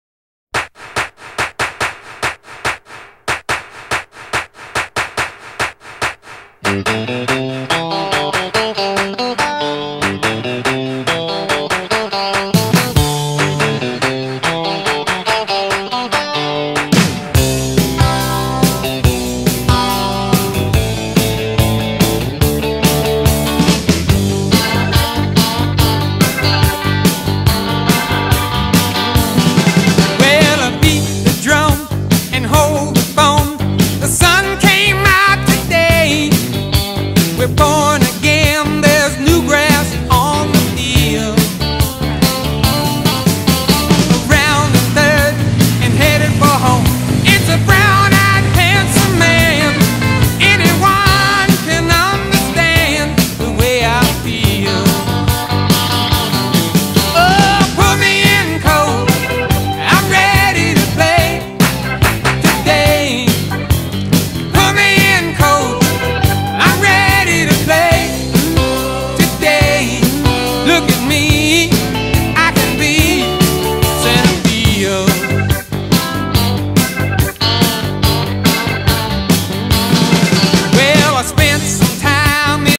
2010 Viterra Western Canadian Midget 'B' Softball Championships
The 2010 Viterra Western Canadian Midget 'B' Boys and Girls Softball Championships were held in the Town of Unity, Saskatchewan, July 30th to August 2nd, 2010.